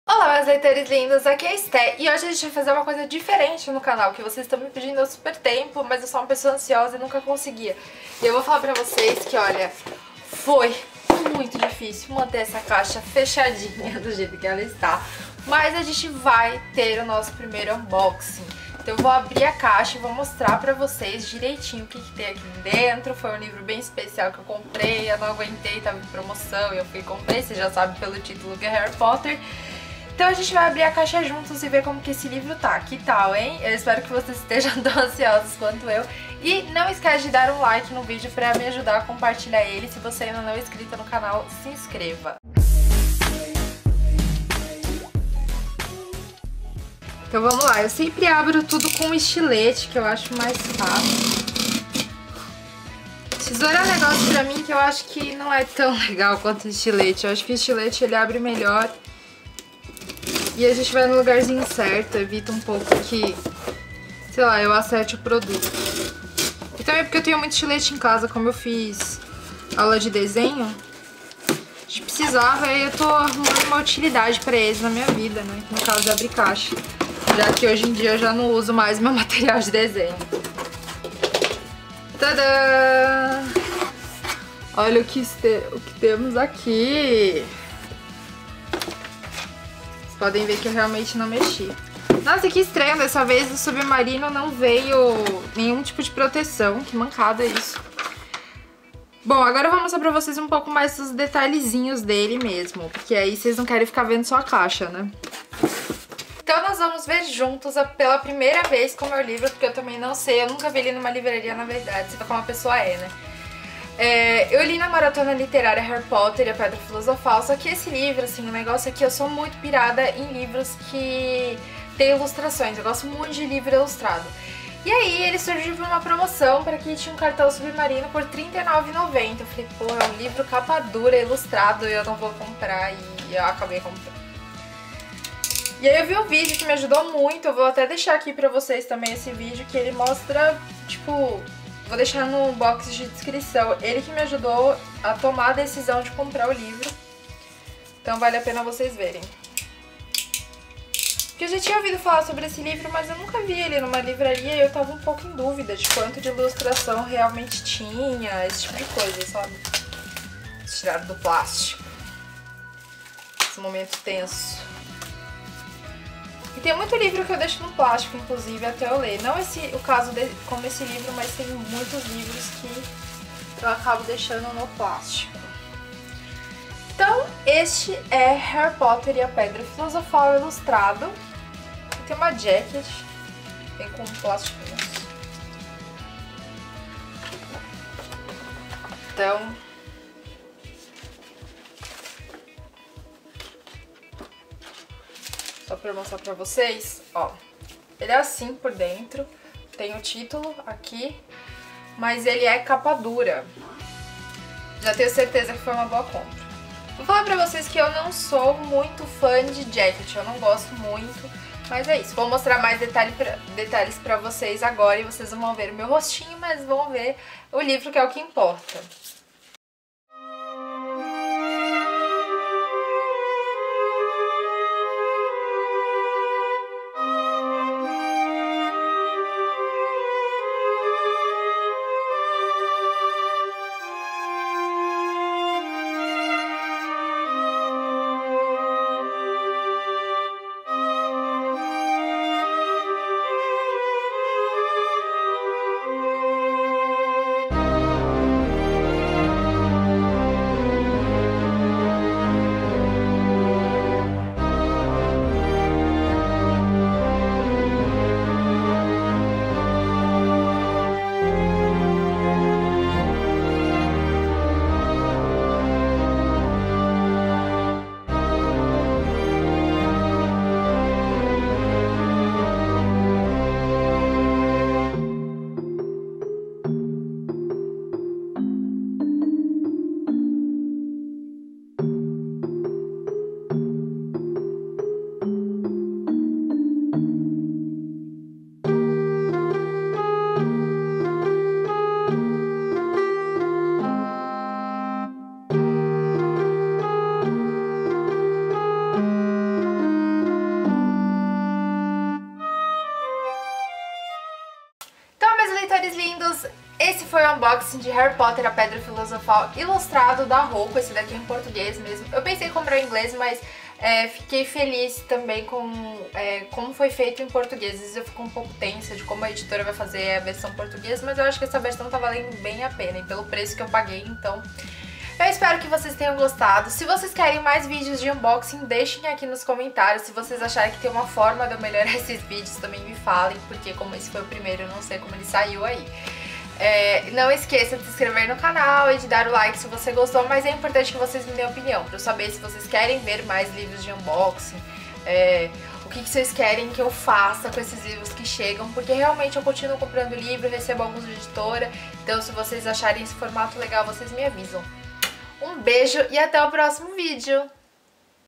Unboxing Harry Potter e a Pedra Filosofal Edição Ilustrada da Rocco | Steh Barbosa
Confira meu primeiro vídeo de unboxing, me conta o que você achou e o que eu posso melhorar! Hoje, vou mostrar para vocês uma quisição recente: Harry Potter e a Pedra Filosofal Edição Ilustrada em português da Editora Rocco.

Quer acompanhar meu dia a dia e fotos legais? @steh_b no insa e livrosderomance no snapchat